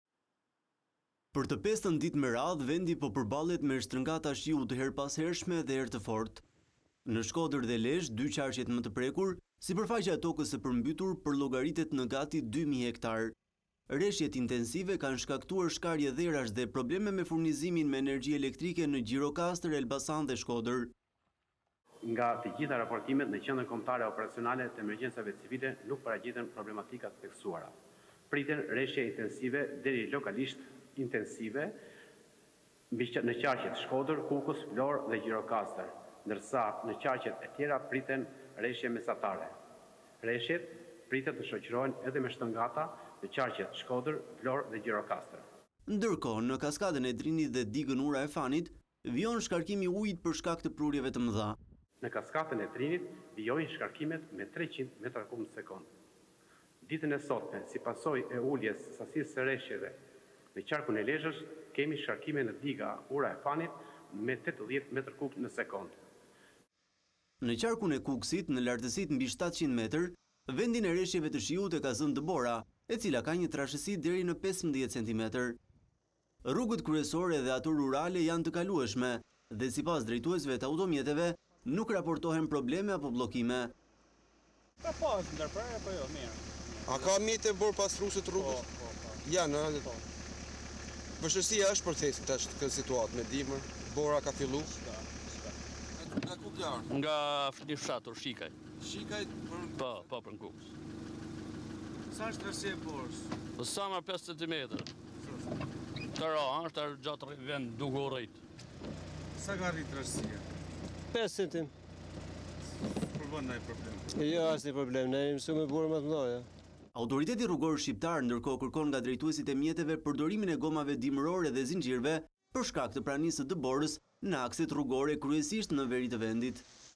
Përmbytje dhe dëborë: Reshjet vijojnë nga veriu në jug, lumenjtë nën pikën kritike
Moti i keq ka shkakuar rreth 2 mijë hektarë tokë e përmbytur në Shkodër dhe Lezhë ndersa ne veri shiu që i ka lënë vendin dëborës.

Agjencia Kombëtare e Mbrojtjes Civile paralajmëron vijim të reshjeve intensive ndërsa në kaskadën e Drinit  vijon shkarkimi i ujit.